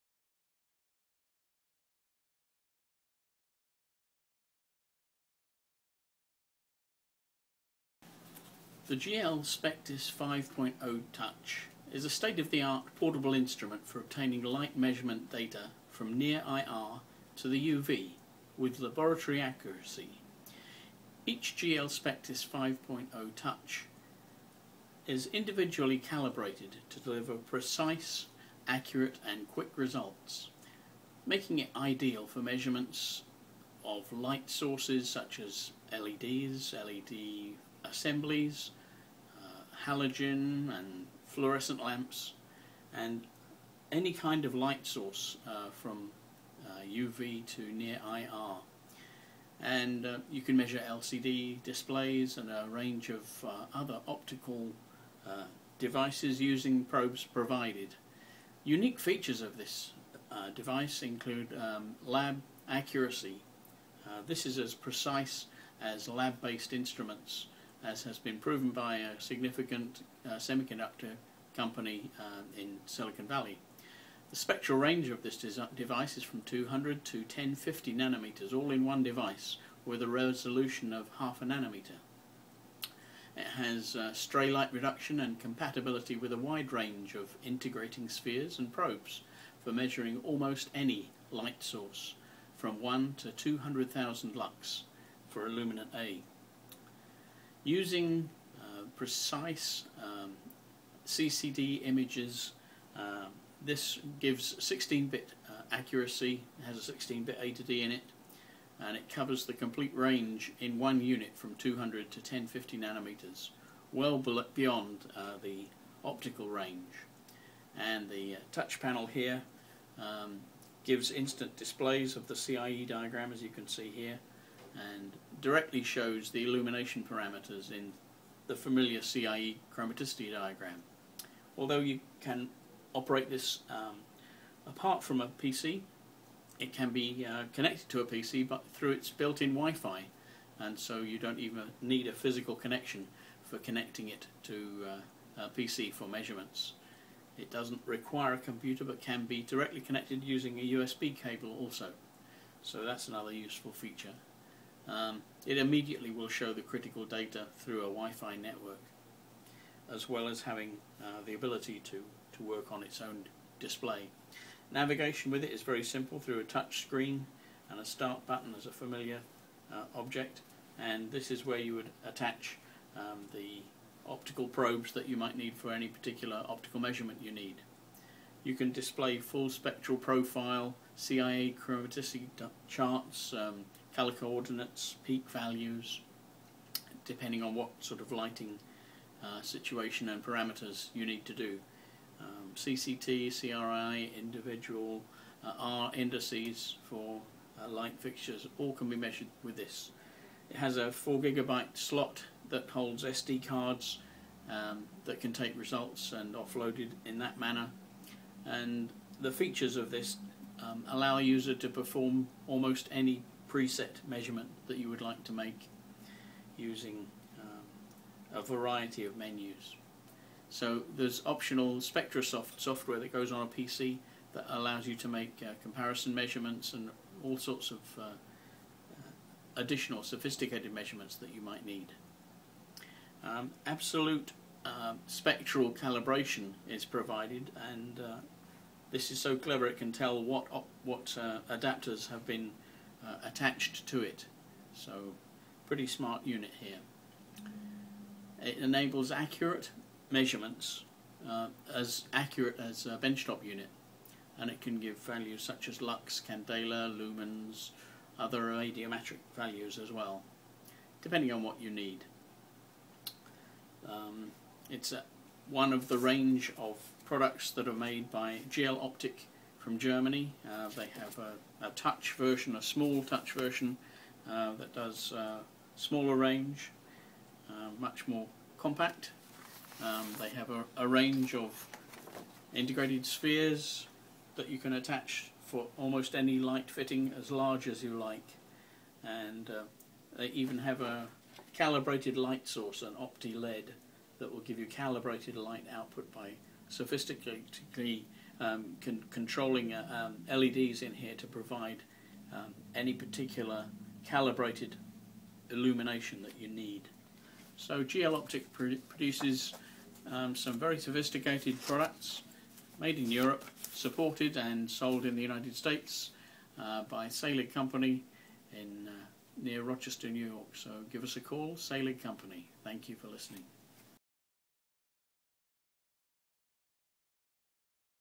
Spectis 5.0 Touch Handheld Spectrophotometer by GL Optic from Saelig Co. Inc.
Calibrated laboratory 200 to 1050nm accuracy in a handheld unit with 0,5nm resolution  for measuring all light sources. OSR system for Stray Light Reduction, various Integrating spheres and probes available, automatic base-line correction.